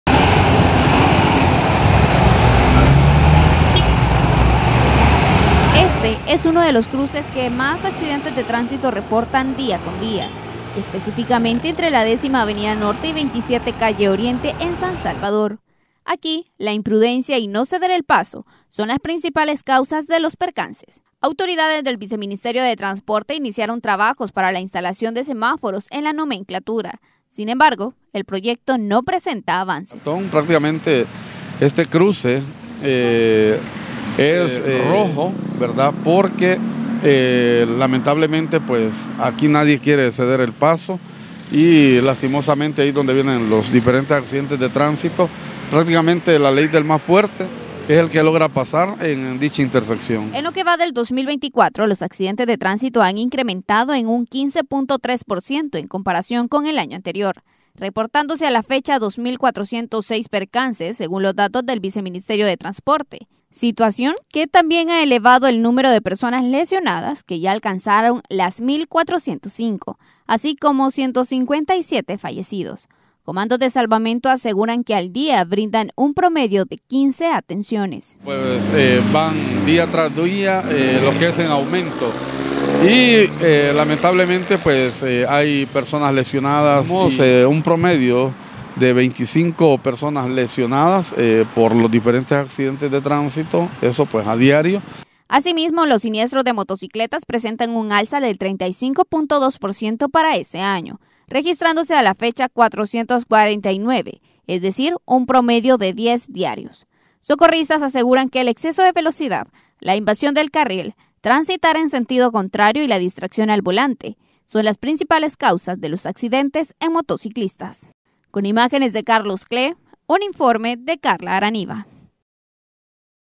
Incremento de accidentes de tránsito
Un promedio de 54 accidentes de tránsito se registra a diario en el país, cifras que representan un incremento del 15.3% en comparación con el año anterior.